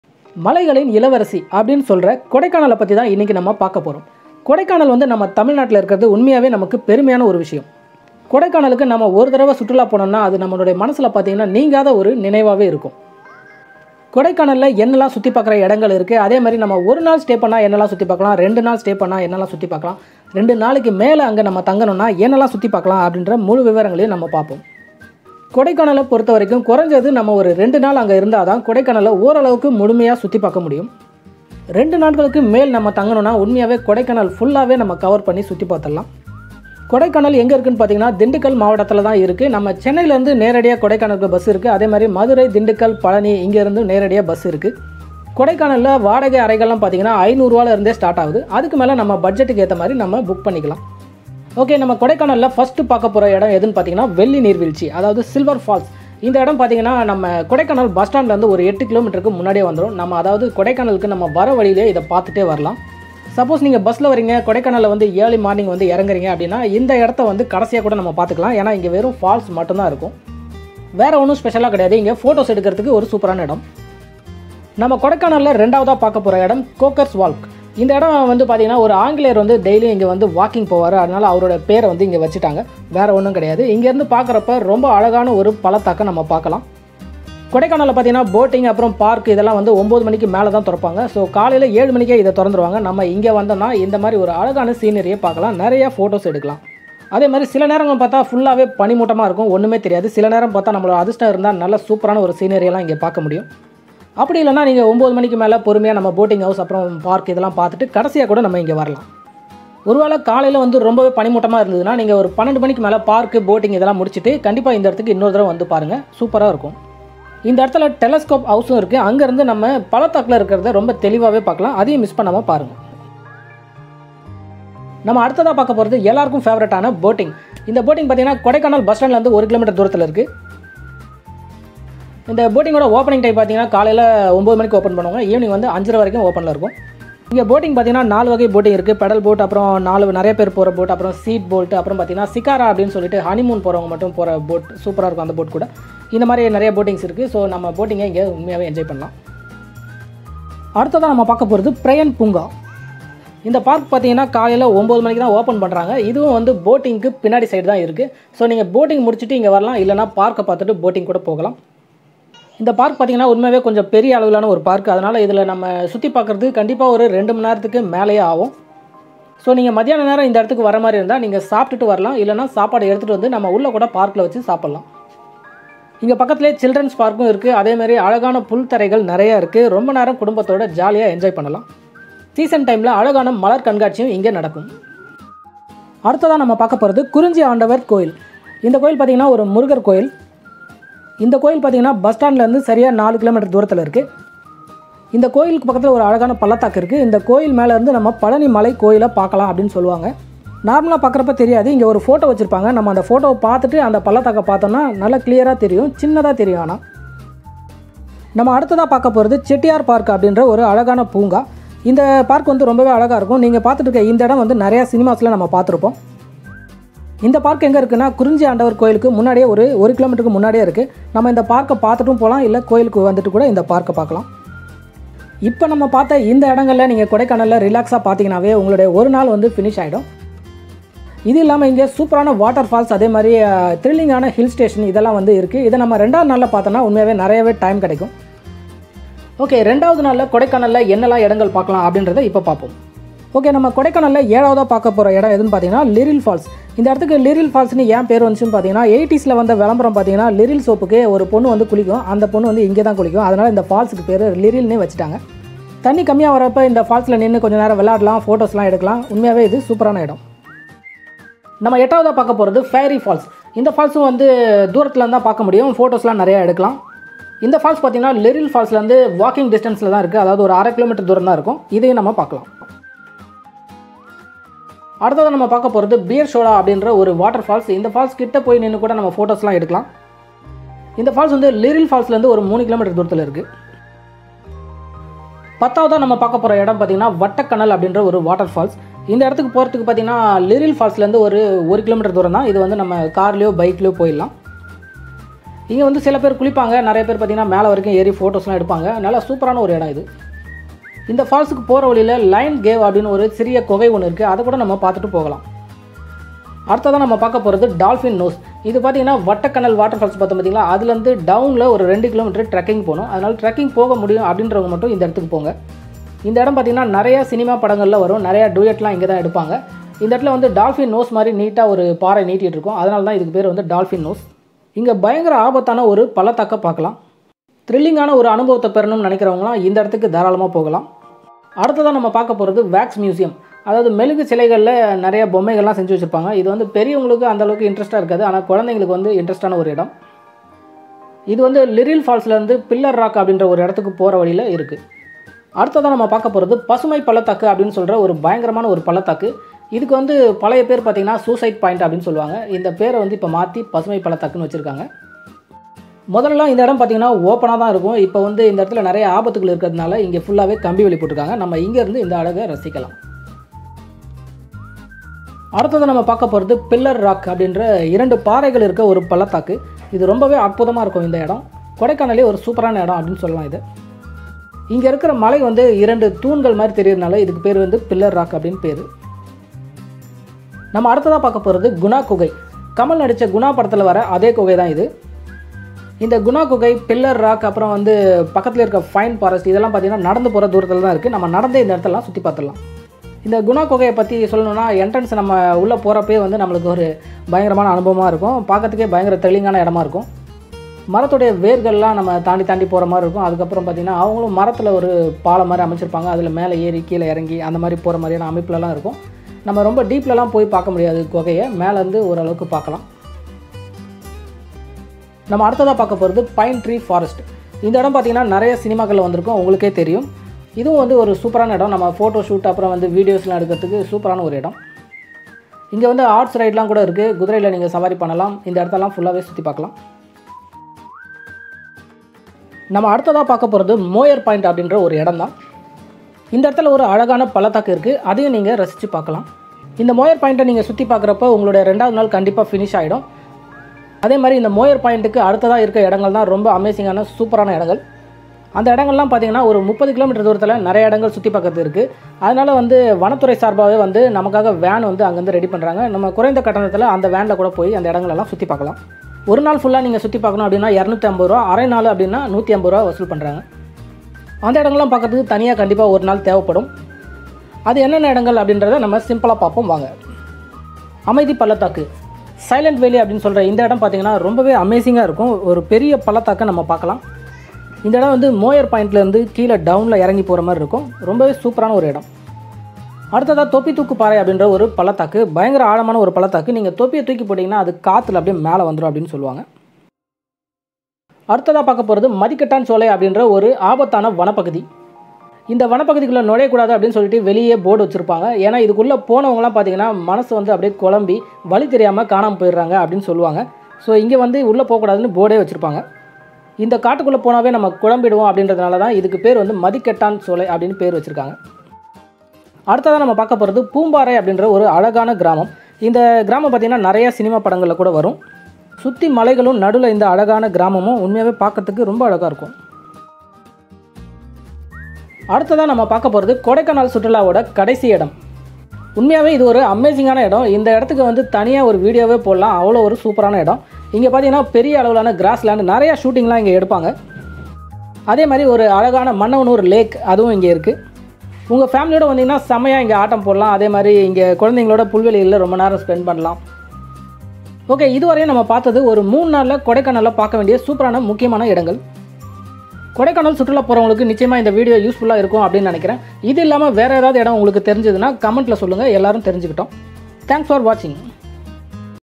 Top 22 Tourist Places Kodaikanal - கொடைக்கானலின் முக்கியமான 22 சுற்றுலா இடங்கள்
கொடைக்கானல் சுற்றுலா போறீங்களா...
இந்த வீடியோவை பார்த்துட்டு போங்க👍
கொடைக்கானலில் 22.முக்கியமான சுற்றுலா இடங்கள்🙌🙌